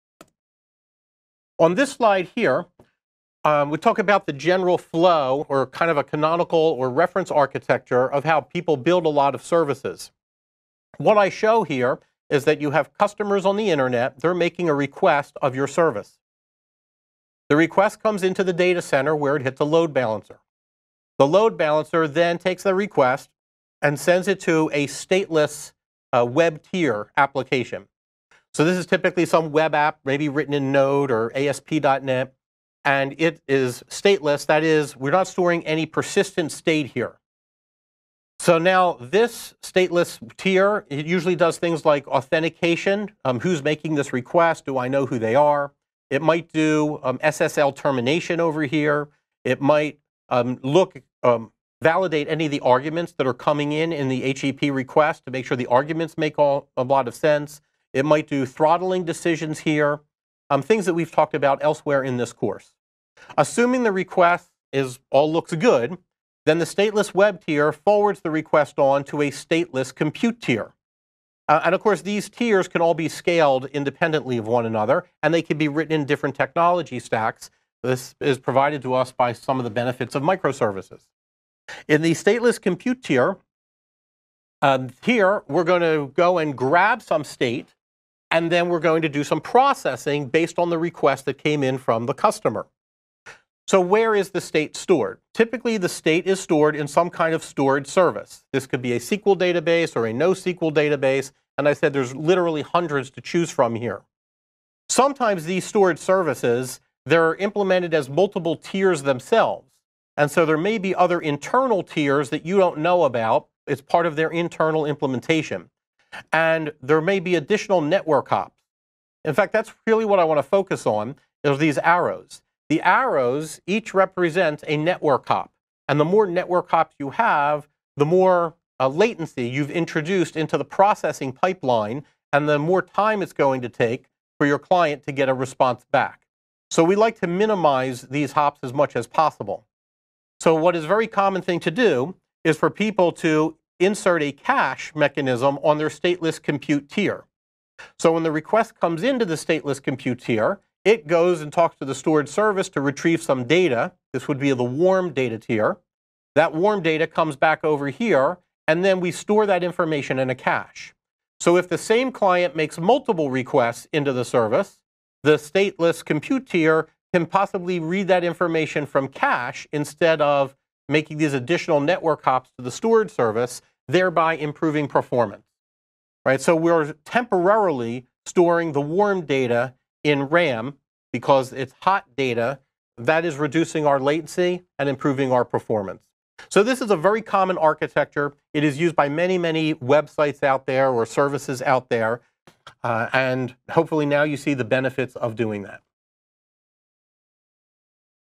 6.1.c-Data Storage--Fundamentals--Using a cache to improve performance
This video discusses the typical topology of a an app that uses multiple stateless services and a back-end storage service. And, to improve performce, you can introduce a cache.